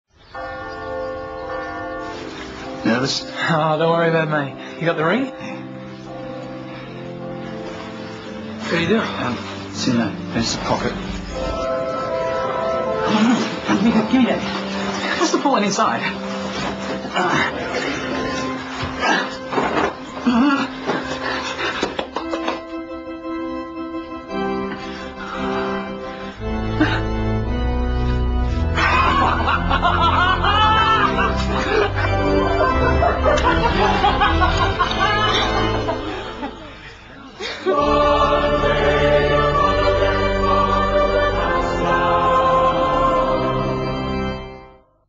The Lost Wedding RIng
This guy is about to get married but his Best man can't find the ring...very hilarious...jus watch